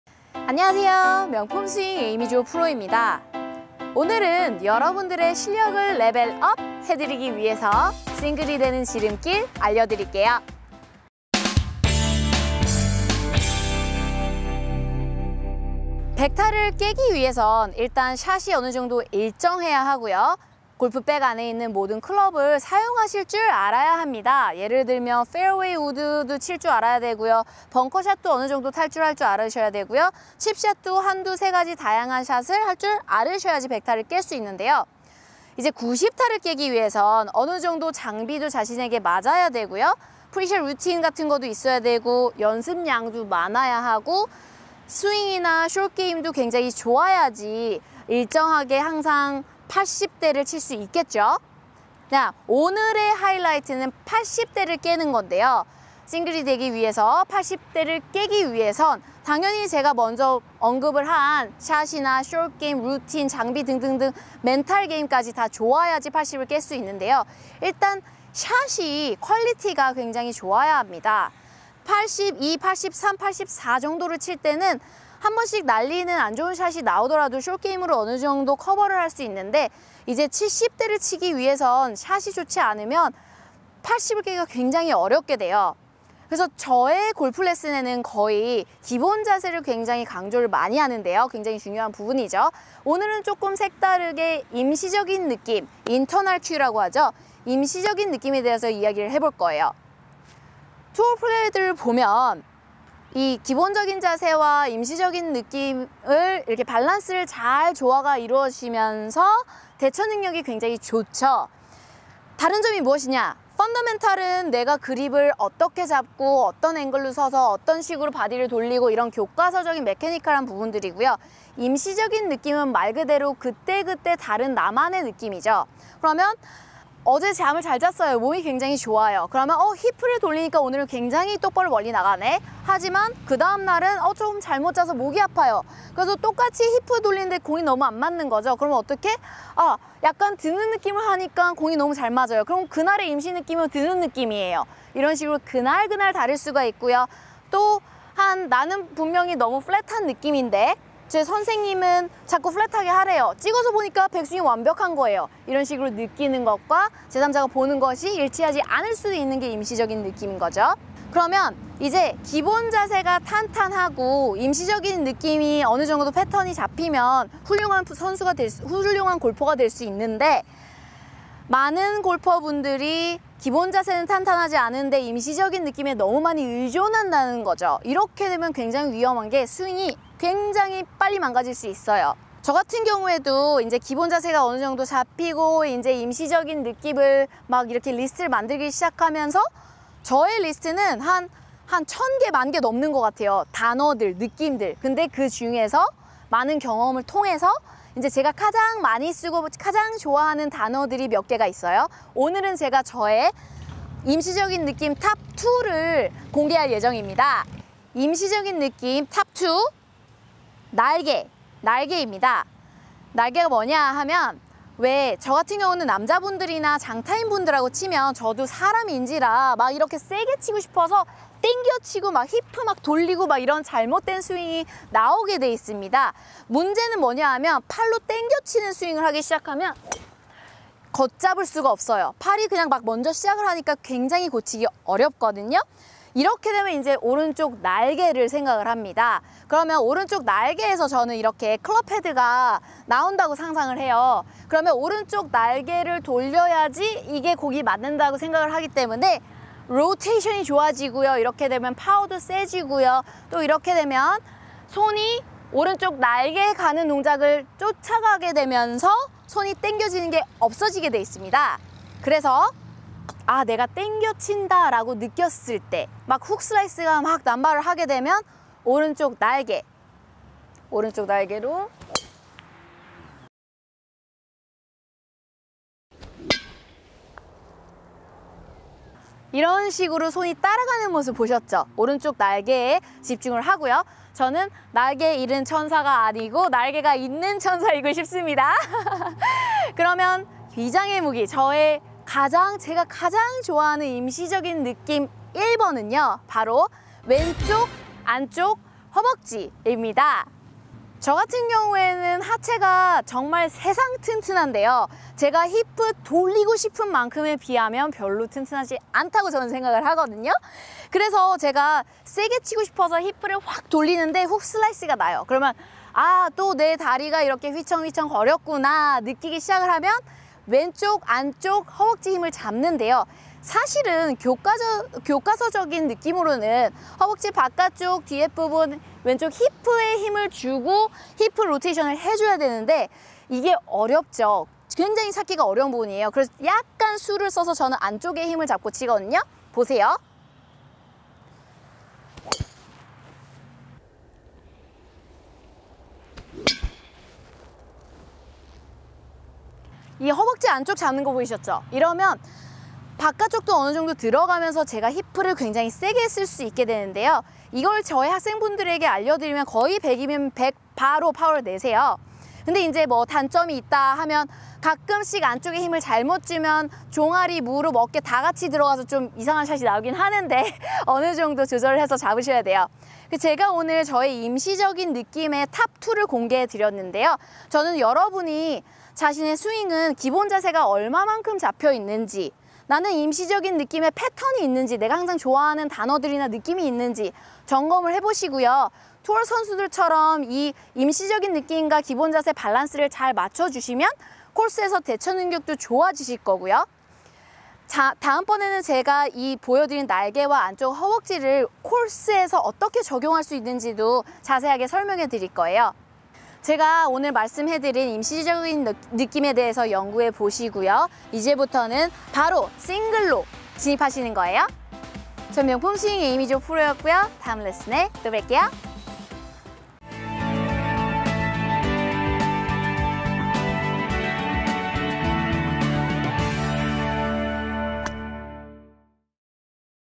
싱글 되는 노하우 | 명품스윙 에이미 조
80대를 깨기 위한 에이미 만의 노하우!
이 비밀만 알면 시간 절약, 레슨비 절약, 에너지 절약 하며 실력 향상!
싱글 핸디캡을 원하시는분들 채널 고정! 어떻게 하면 가장 효과적으로 80대를 깰수있는지 알려드릴게요.

즐감하셔요!
궁금하신점 있으시면
으로 오셔서 질문해주세요!

영상이 즐거우셨다면 “좋아요” 버튼 눌러 주셔용~
유투브 체널에 구독 버튼 꾹! 누르시고 자동 업데이트 받으세요!

에이미의 SNS를 통해 소통하세요!